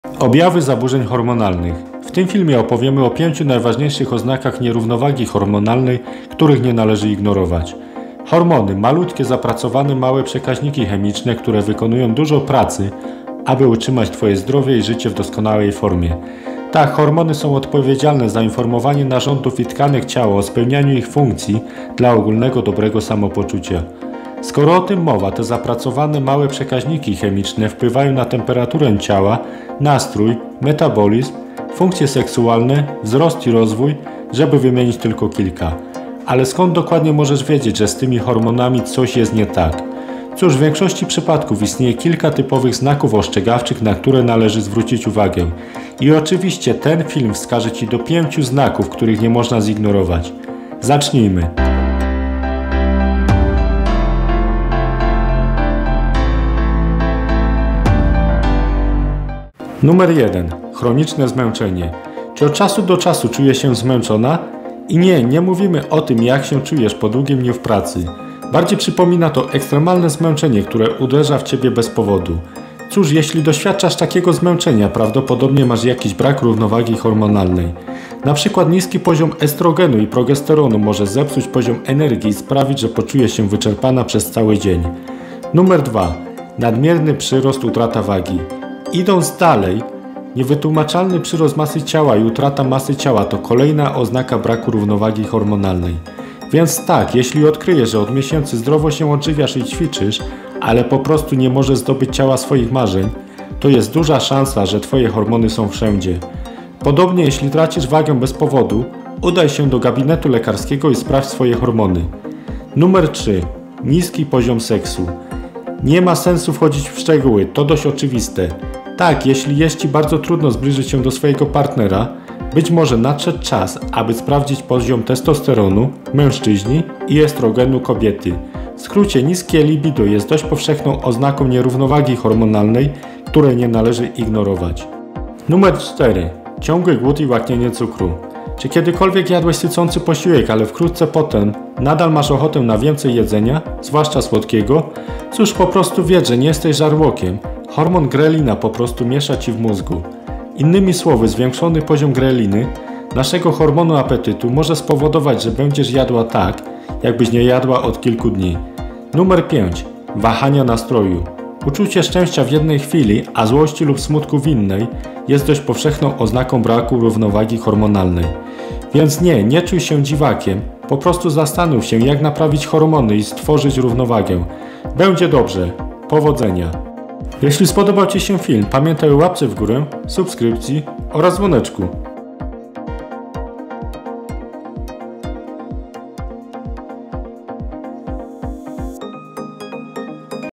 Objawy zaburzeń hormonalnych
W tym filmie opowiemy o 5 najważniejszych oznakach  nierównowagi hormonalnej, których nie należy ignorować.
Hormony — malutkie, zapracowane małe przekaźniki chemiczne, które wykonują dużo pracy, aby utrzymać Twoje zdrowie i życie w doskonałej formie.
Tak, hormony są odpowiedzialne za informowanie narządów i tkanek ciała o spełnianiu ich funkcji dla ogólnego dobrego samopoczucia.
Skoro o tym mowa, te zapracowane małe przekaźniki chemiczne wpływają na temperaturę ciała, nastrój, metabolizm, funkcje seksualne, wzrost i rozwój, żeby wymienić tylko kilka.
Ale skąd dokładnie możesz wiedzieć, że z twoimi hormonami coś jest nie tak?
Cóż, w większości przypadków istnieje kilka typowych znaków ostrzegawczych, na które należy zwrócić uwagę. I oczywiście ten film wskaże Ci do 5 znaków, których  nie można zignorować.
Zacznijmy!
1. Chroniczne zmęczenie
Czy od czasu do czasu czujesz się zmęczona?
I nie, nie mówimy o tym, jak się czujesz po długim dniu w pracy.
Bardziej przypomina to ekstremalne zmęczenie, które uderza w ciebie bez powodu.
Cóż, jeśli doświadczasz takiego zmęczenia, prawdopodobnie masz jakiś brak równowagi hormonalnej.
Na przykład niski poziom estrogenu i progesteronu może zepsuć poziom energii i sprawić, że poczujesz się wyczerpana przez cały dzień.
2. Nadmierny przyrost/utrata wagi
Idąc dalej, niewytłumaczalny przyrost masy ciała i utrata masy ciała to kolejna oznaka braku równowagi hormonalnej.
Więc tak, jeśli odkryjesz, że od miesięcy zdrowo się odżywiasz i ćwiczysz, ale po prostu nie możesz zdobyć ciała swoich marzeń, to jest duża szansa, że ​​twoje hormony są wszędzie.
Podobnie, jeśli tracisz wagę bez powodu, udaj się do gabinetu lekarskiego i sprawdź swoje hormony.
3. Niski poziom seksu
Nie ma sensu wchodzić w szczegóły — to dość oczywiste.
Tak, jeśli jest Ci bardzo trudno zbliżyć się do swojego partnera, być może nadszedł czas, aby sprawdzić poziom testosteronu (mężczyźni) i estrogenu (kobiety).
W skrócie; niskie libido jest dość powszechną oznaką nierównowagi hormonalnej, której nie należy ignorować.
4. Ciągły głód i łaknienie cukru
Czy kiedykolwiek jadłeś sycący posiłek, ale wkrótce potem nadal masz ochotę na więcej jedzenia (zwłaszcza słodkiego)?

Cóż, po prostu wiedz, że nie jesteś żarłokiem — hormon grelina po prostu miesza ci w mózgu.
Innymi słowy, zwiększony poziom greliny (naszego hormonu apetytu) może spowodować, że będziesz jadła tak, jakbyś nie jadła od kilku dni.
5. Wahania nastroju
Uczucie szczęścia w jednej chwili, a złości lub smutku w innej, jest dość powszechną oznaką braku równowagi hormonalnej.
Więc nie, nie czuj się dziwakiem — po prostu zastanów się, jak naprawić hormony i stworzyć równowagę.
Będzie dobrze!
Powodzenia!